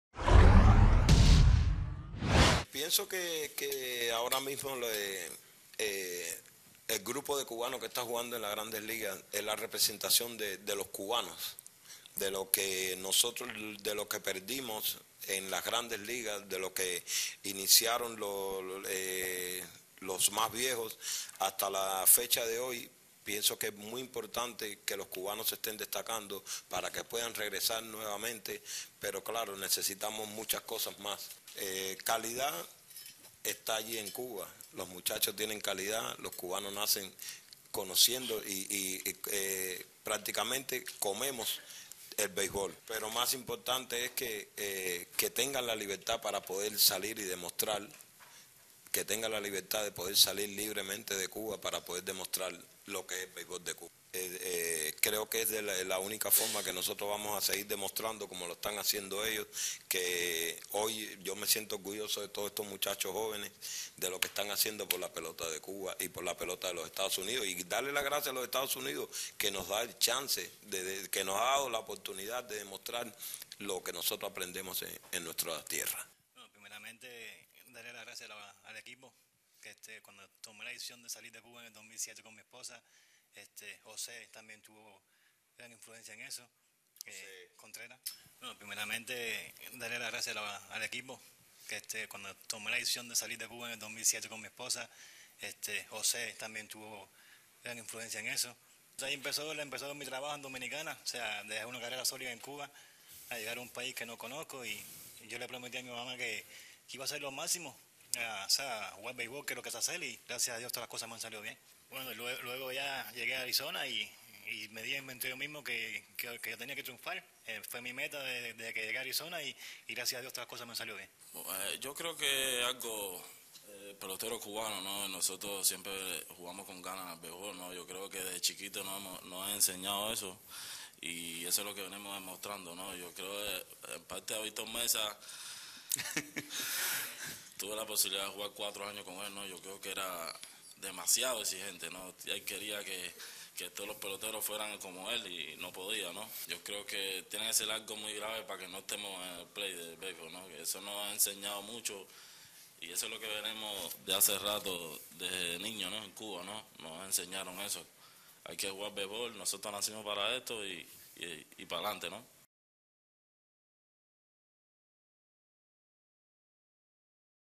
Hernandez, Ramirez, Viciedo sobre Cubanos en Beisbol
Orlando Hernandez, Alexei Ramirez y Dayan Viciedo participan en una discusion sobre la vida y el beisbol Cubano